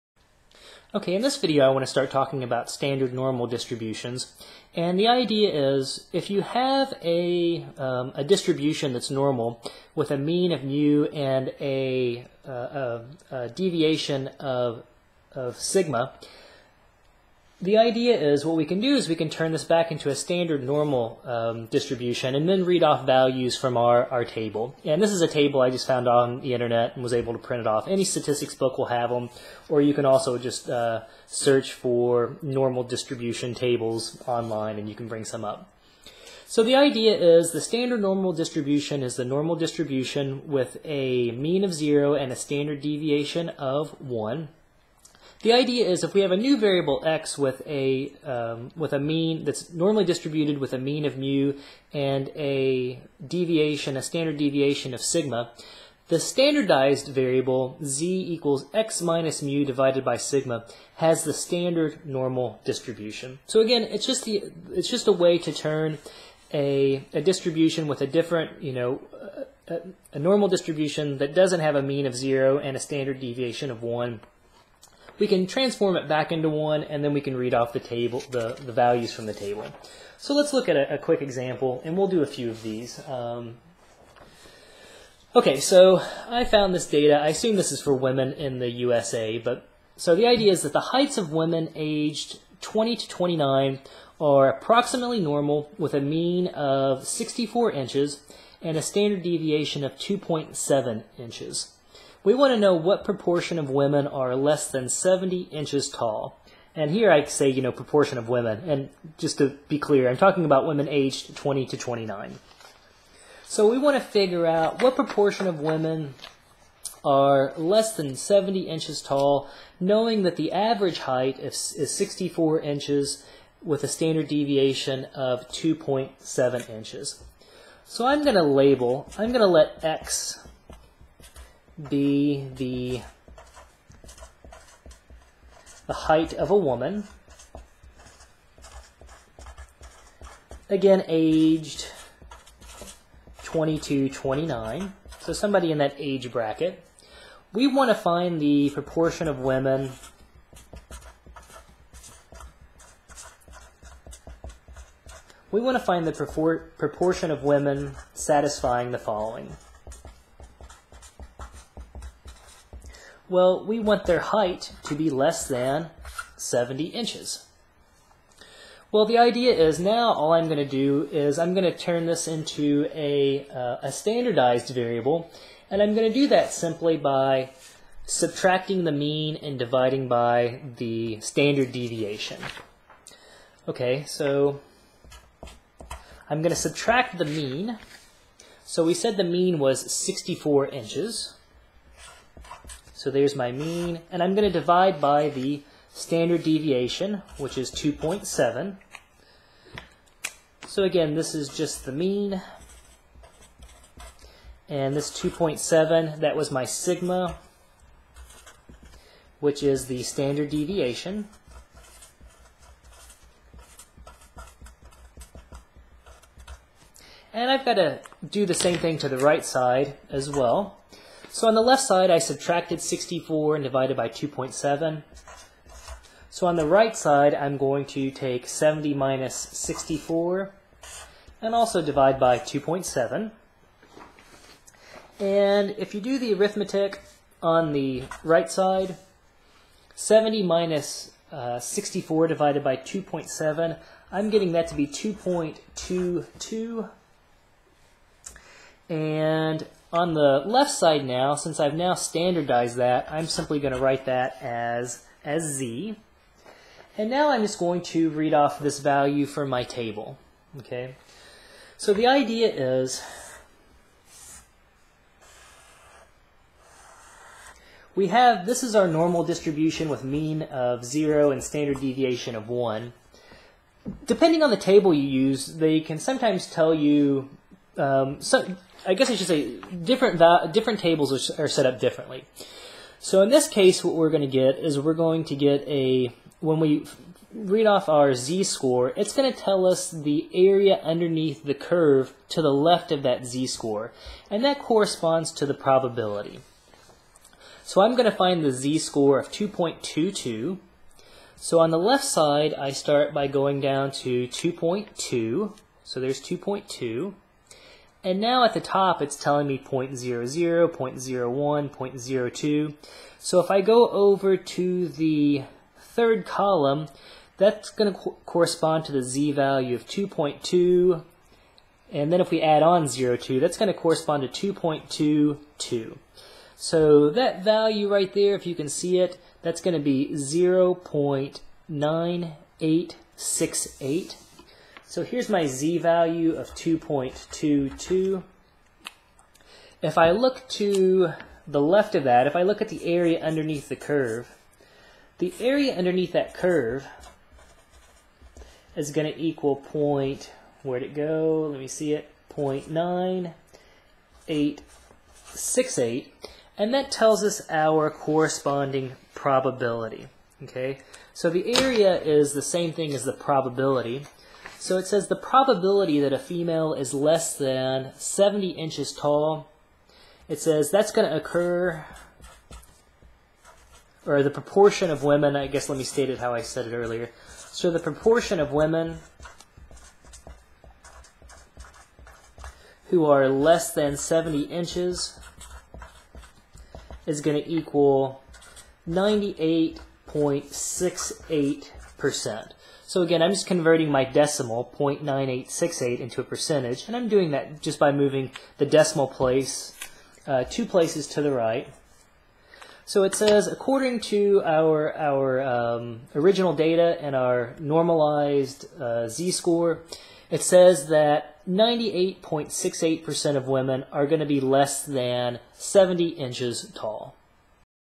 Normal Distribution and Z-scores - Example 1
Here we look at how to standardize a normal distribution so that we can read values using z-scores and standard normal table.